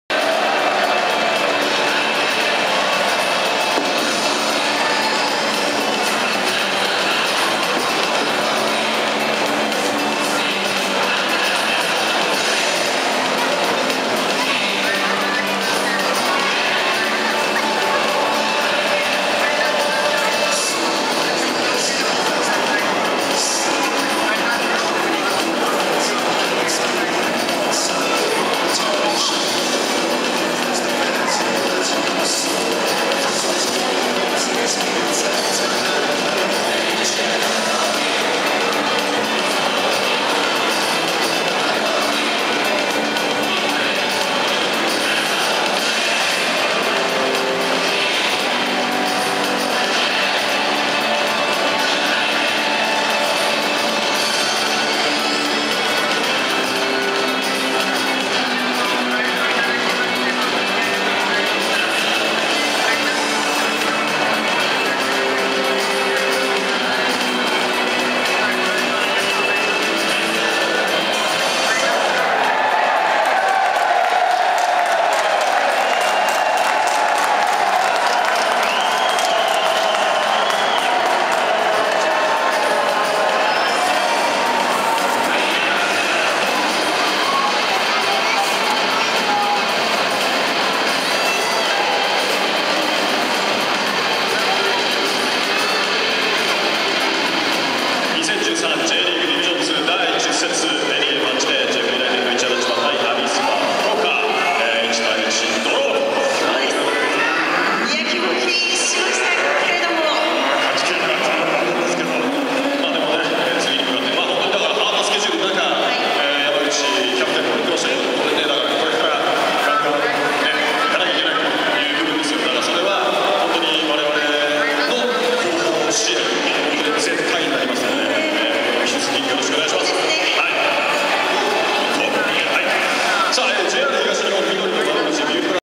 ジェフ千葉'13 vs福岡@フクアリ 試合後ダイジェスト映像(途中から) 21,Apr,2013
試合後のダイジェスト映像(途中から) と
ジェフサポーターの歓声

2013 Jリーグ Division2  第10節
ジェフユナイテッド市原千葉
1-1
アビスパ福岡
JEF's Goal：山口智！ 93'
アシスト 田中佑昌！
@フクダ電子アリーナ(千葉県千葉市)